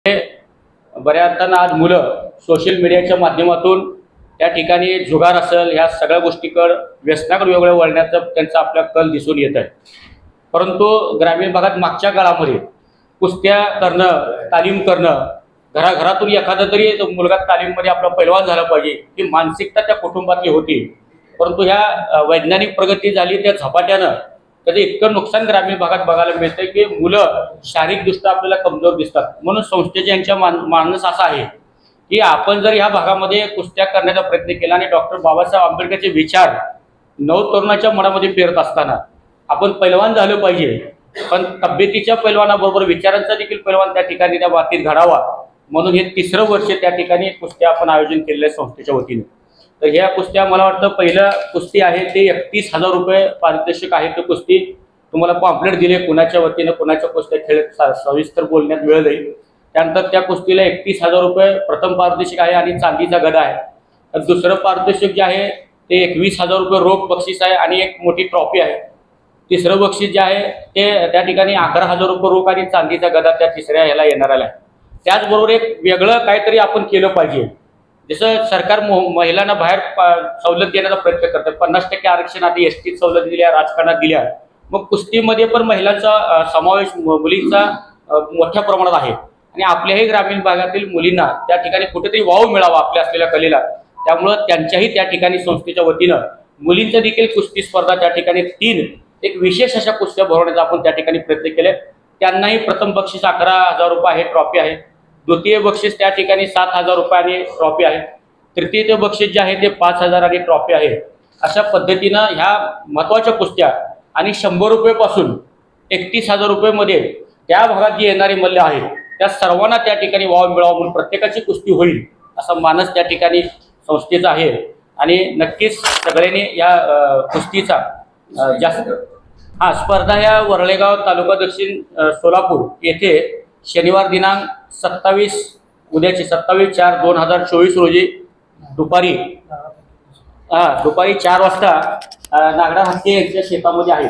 सोलापुरात भव्य जंगी कुस्ती स्पर्धेचे आयोजन..! पहा
क्रांती बहुउद्देशीय मागासवर्गीय सामाजिक संस्थेचा उपक्रम
वरळी गाव येथे नामवंत मल्लांच्या कुस्त्या